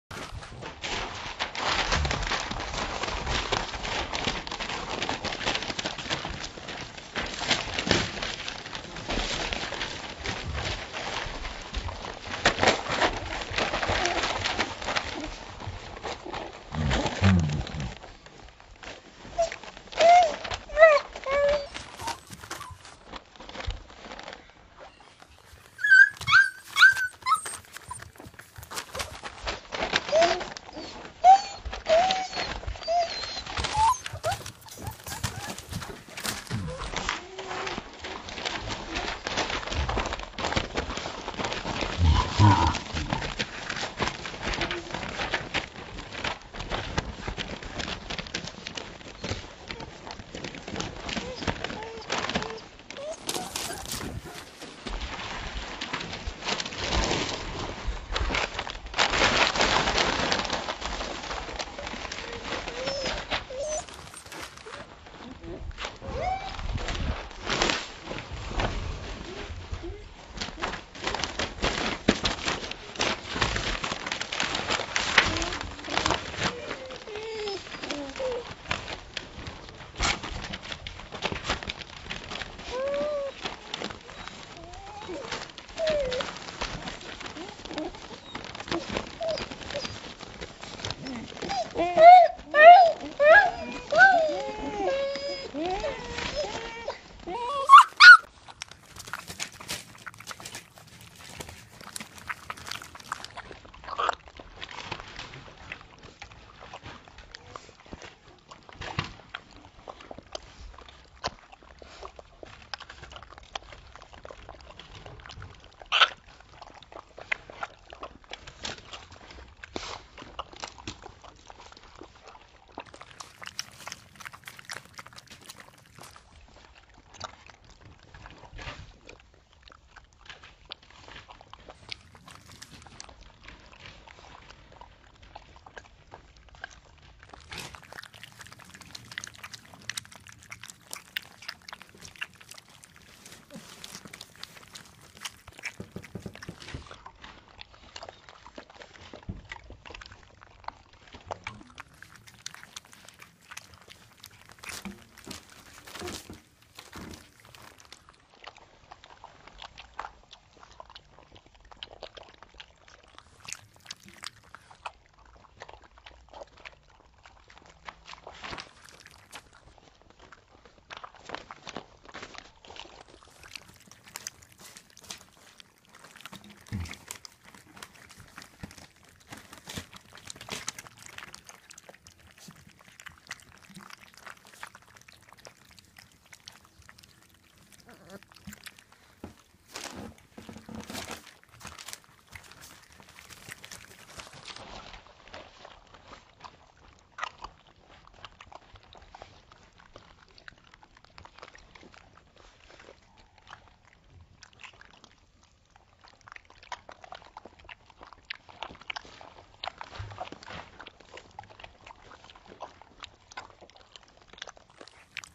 3+ Minutes of Cute Puppies
These gorgeous Rottweiler-Aussie Shepherd mix pups need foster homes for just about two weeks.